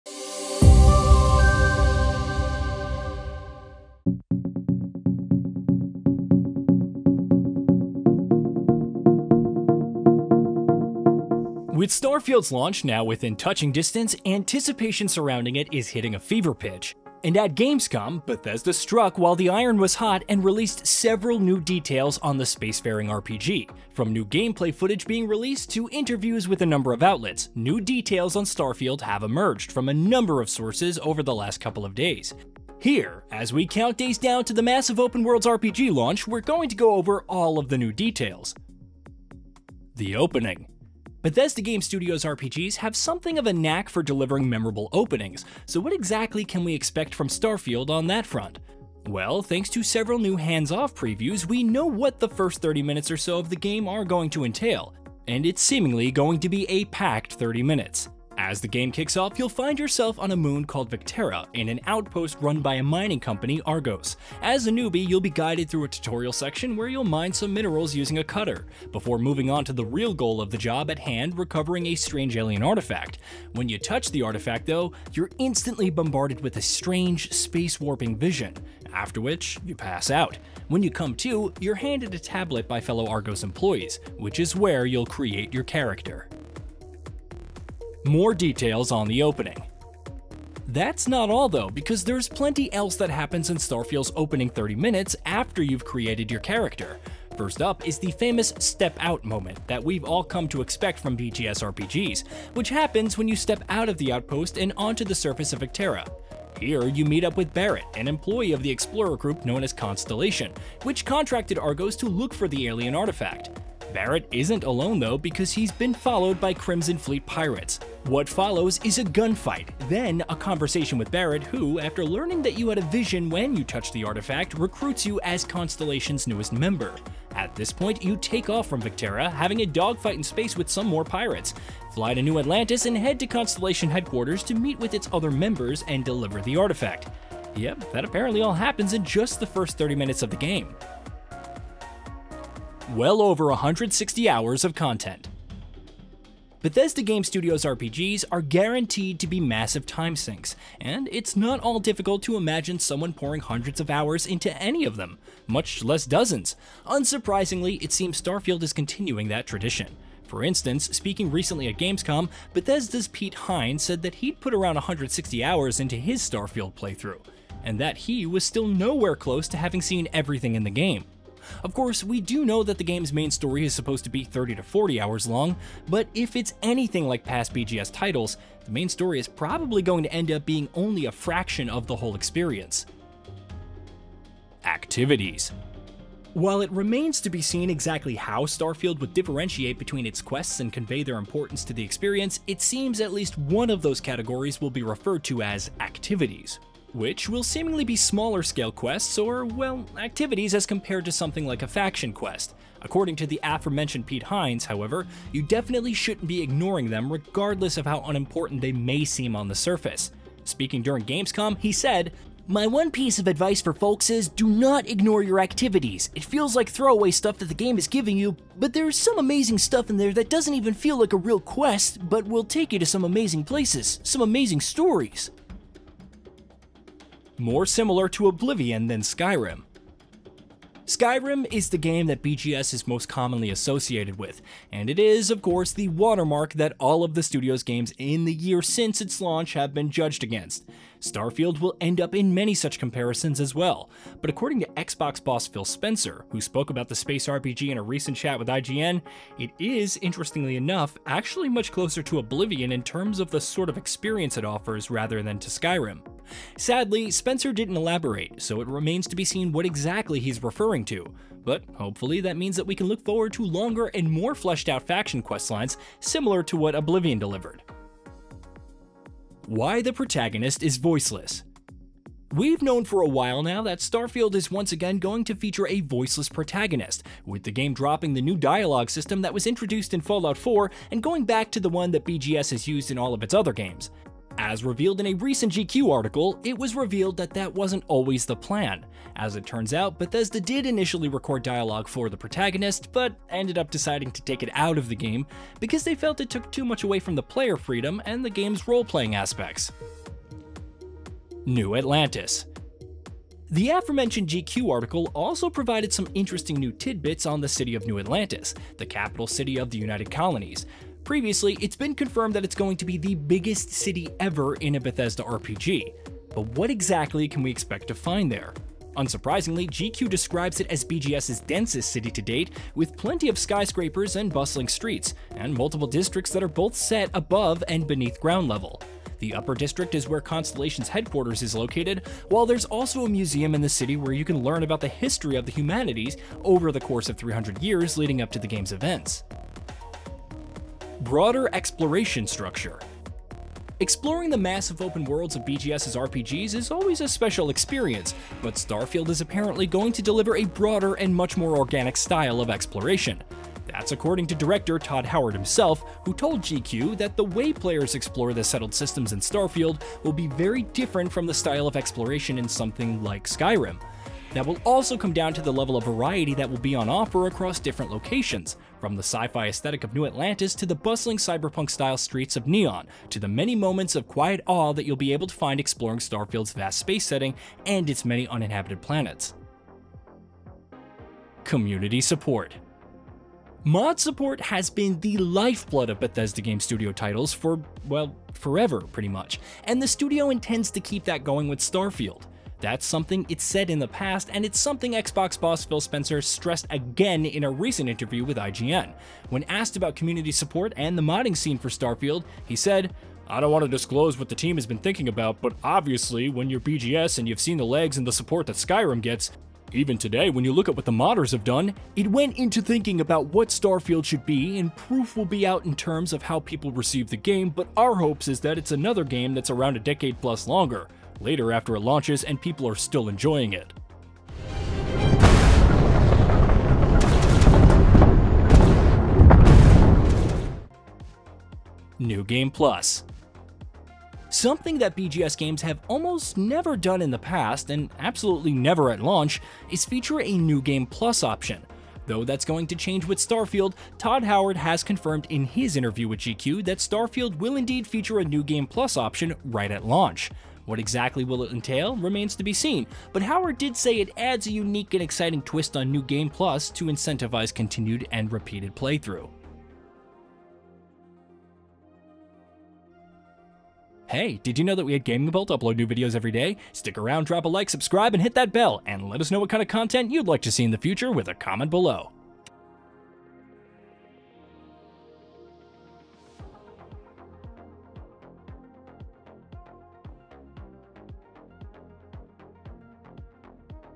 STARFIELD - 10 MORE NEW Details You Need To Know Before You Buy
As we get closer and closer to Starfield’s imminent launch, excitement and anticipation surround its release continues to mount by the day- which, given the fact that it’s a brand new open world RPG by the legendary Bethesda Game Studios, comes as no surprise.

Over the last few days, courtesy of plenty of new hands-off previews, interviews, gameplay showings, and more coming from Gamescom, several new details have emerged on the upcoming space-faring RPG, from behind the scenes development tidbits to interesting new details about what the experience itself will have in store. Here, as we mark off the days on the calendar in anticipation of Starfield’s release, we’re going to go over all of those new details that you should know about.